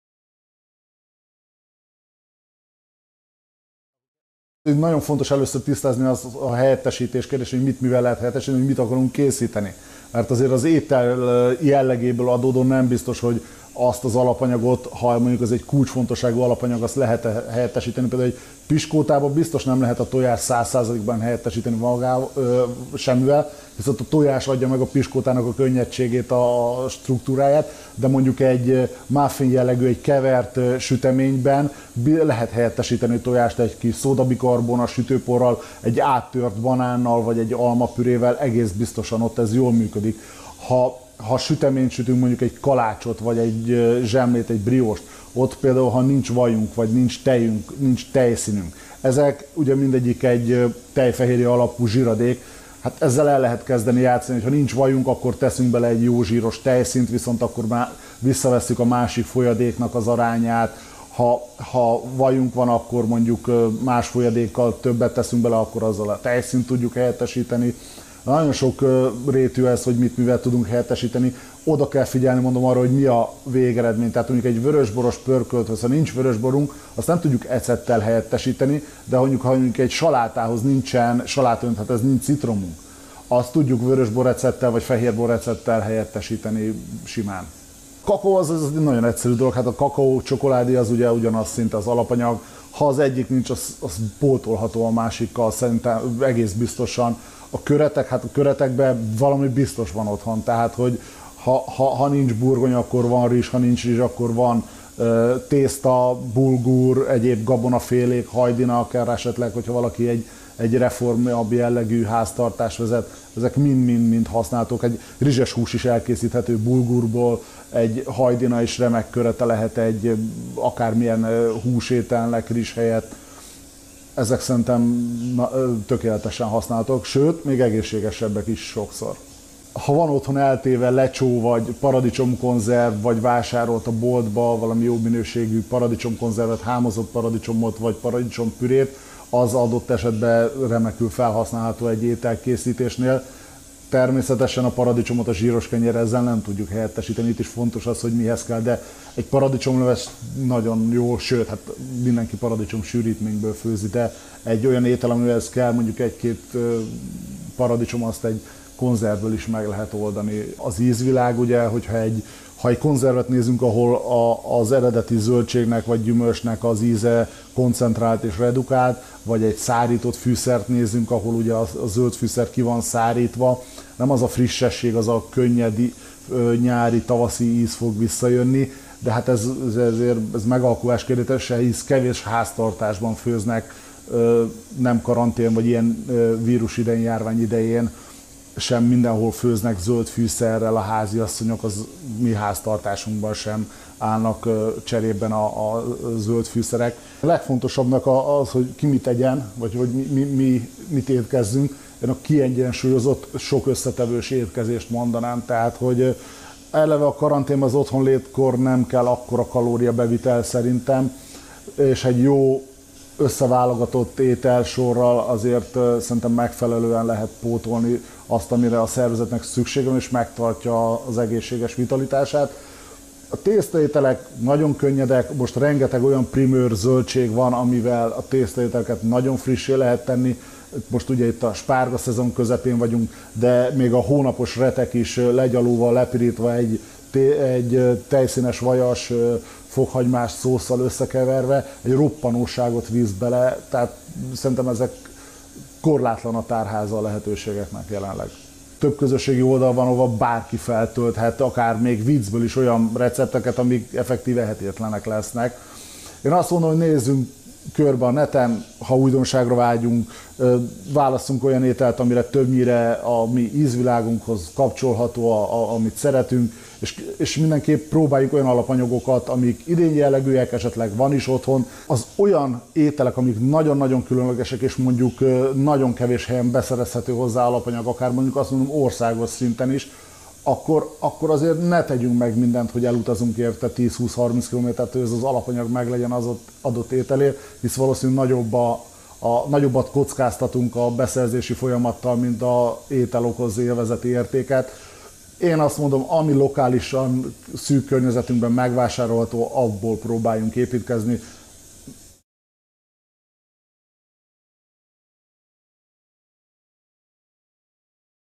Kommentár Nélkül - 2020.04.27. - Alapanyagok helyettesítése
Ezek az alapanyagok helyettesíthetők a konyhában.